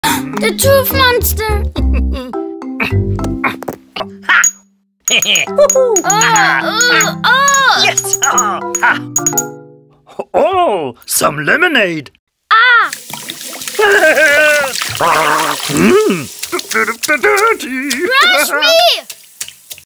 Brush me - Brush your teeth - Dental Health - Kindergarten - Healthy Habits - Easy English
Oh, no! Now the Tooth Monster is coming. "Brush me!" Who saves the dirty Tooth?